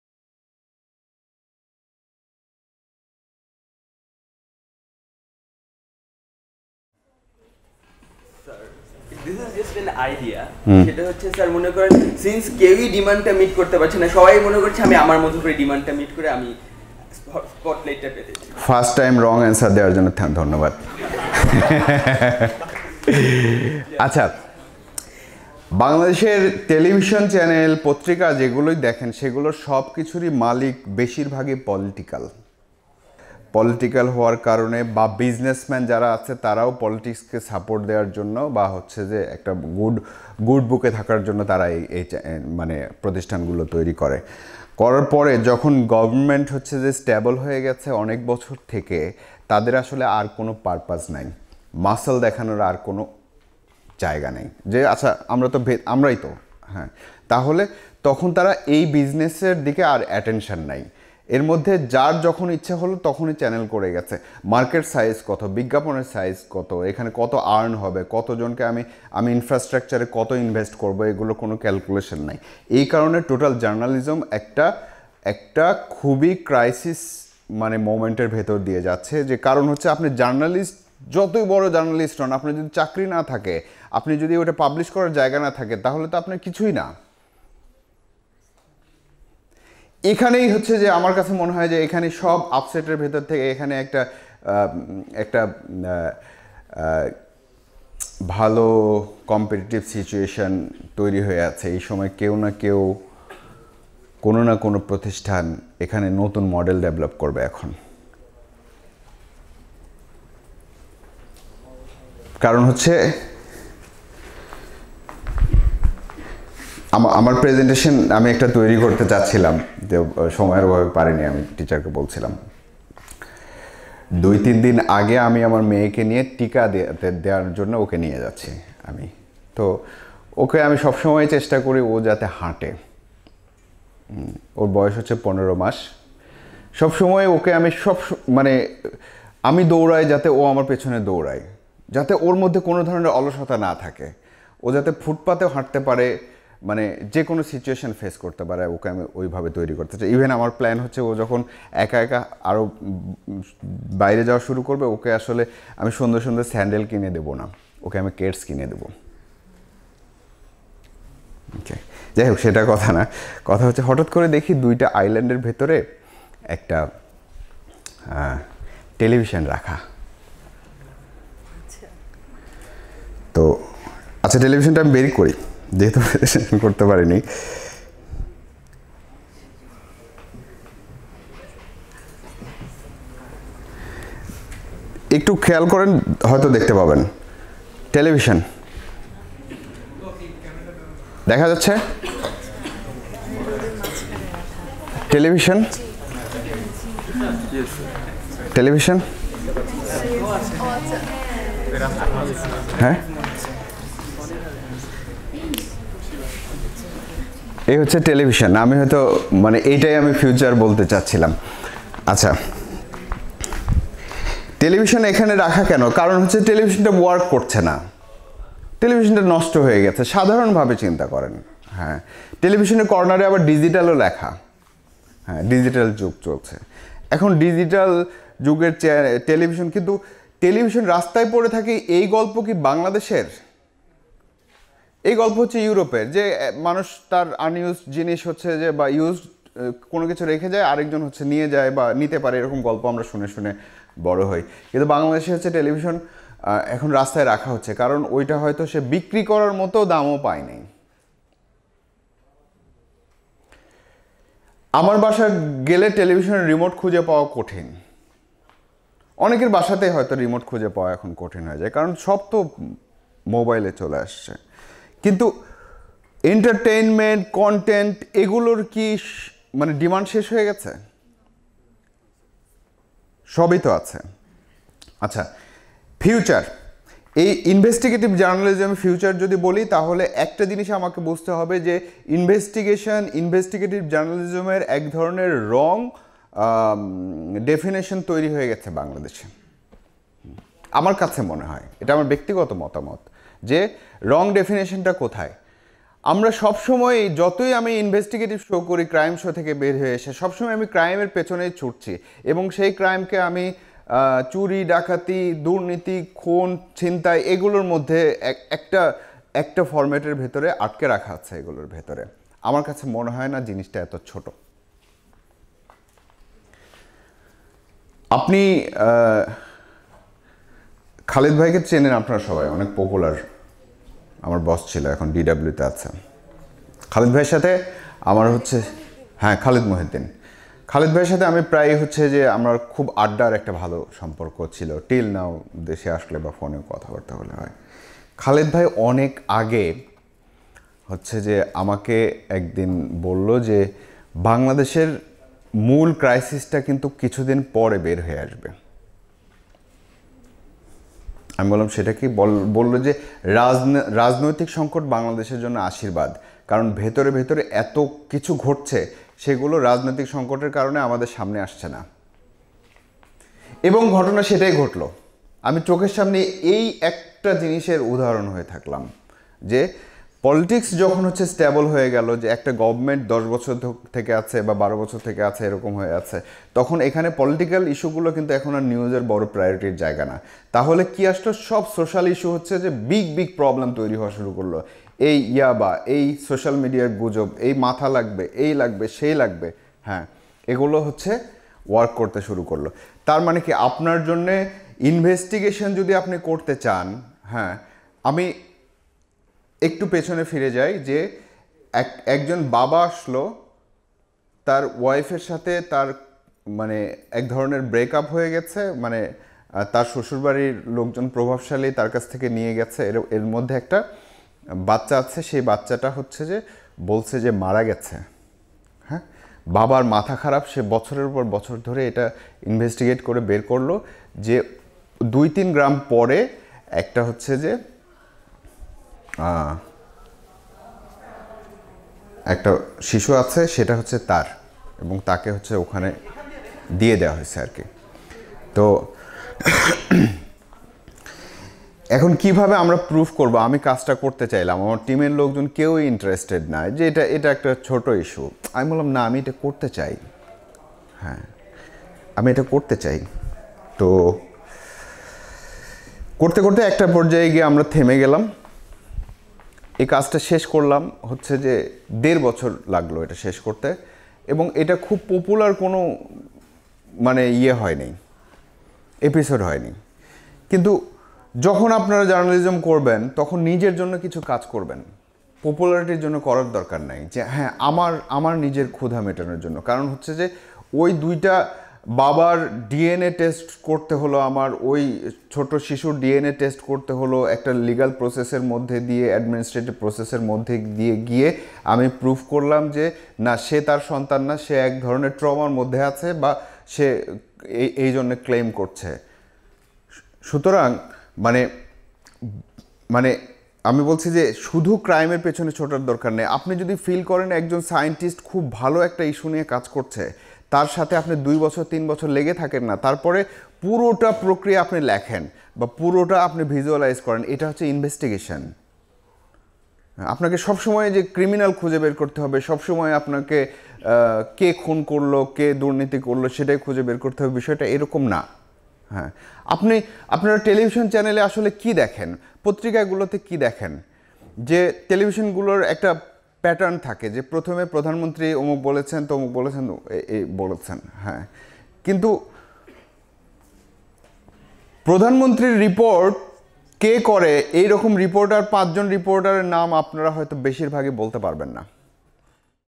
A workshop with Munzurul Karim | Part_05 | Bijoy auditorium
Meet the Journalist A workshop with Munzurul_Karim on BUP Bijoy auditorium

organized by: MCJ Batch 19